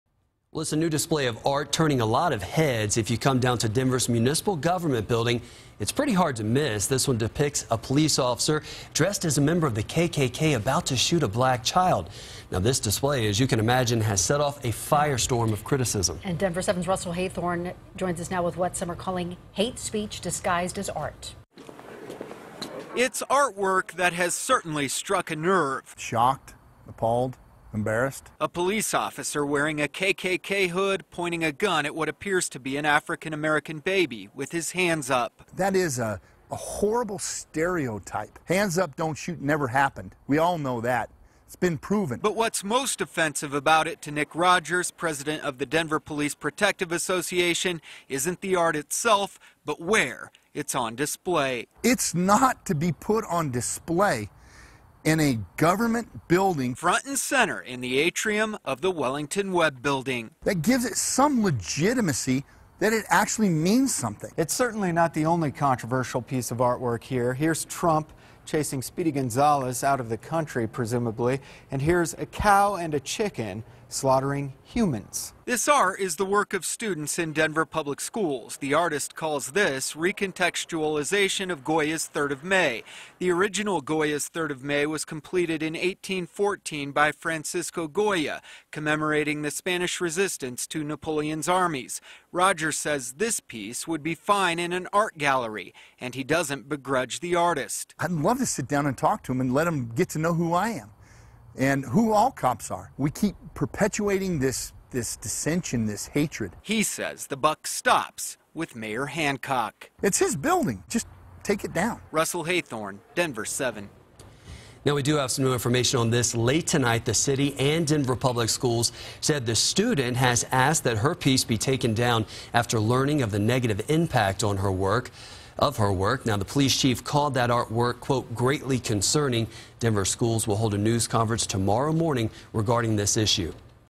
KKK artwork in Denver's Wellington Webb Municipal Office Building comes under fire
A drawing depicting a white police officer as a member of the KKK, pointing a gun at a black child, with a Confederate flag breaking through the American flag is being dubbed 'hate art' by police unions in Colorado.

It's just one of dozens of pieces of artwork currently displayed inside the City of Denver’s Wellington Webb Municipal Office Building.

It has set off a firestorm of criticism.

The National Latino Peace Officers Association is calling it, "hate art." The Denver Police Protective Association i